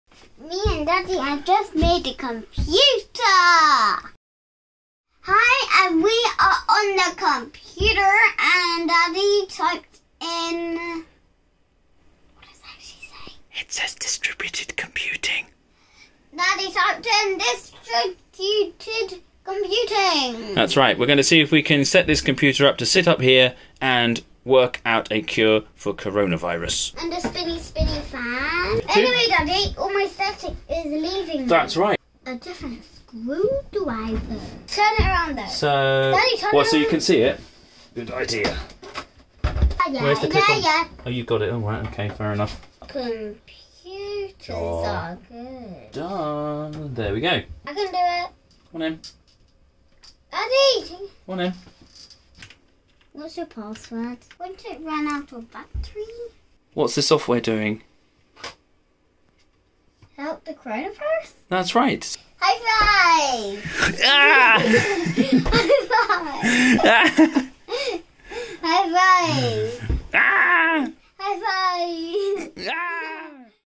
TEASER: COVID-19 Distributed Computing (Highlights with Bryn)
We'll be doing quirky and innovative build guides and walkthroughs, but we've taken time out from that to kick off with some Folding@Home to help the Coronavirus effort. My son Bryn got really into this!